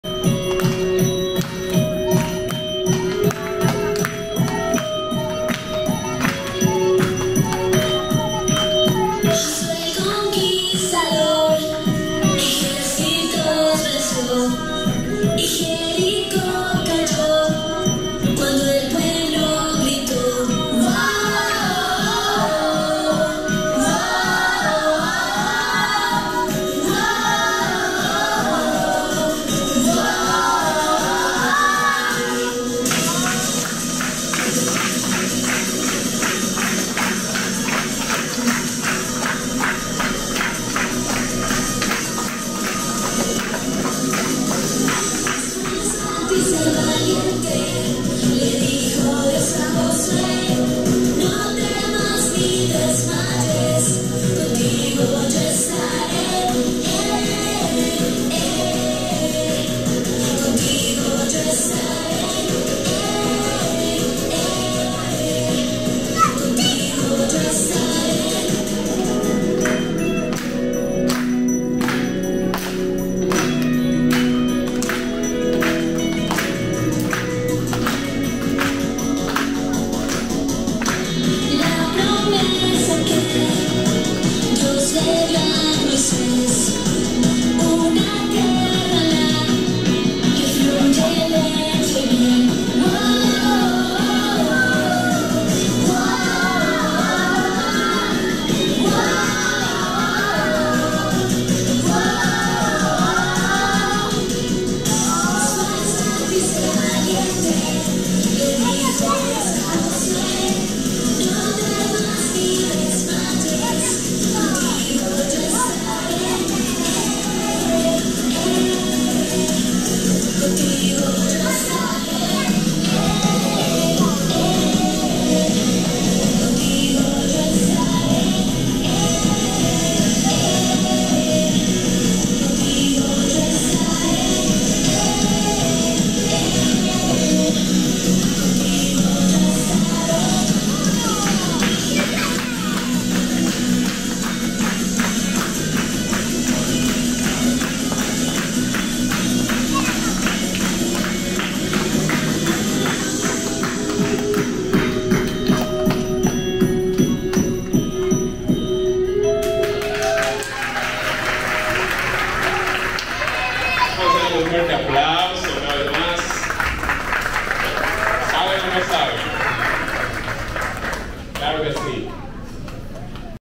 La reacción de niños graduandos en el Colegio Mary Lithgow de Puerto Plata
Estos niños que pasan al primer grado se gozaron su graduación.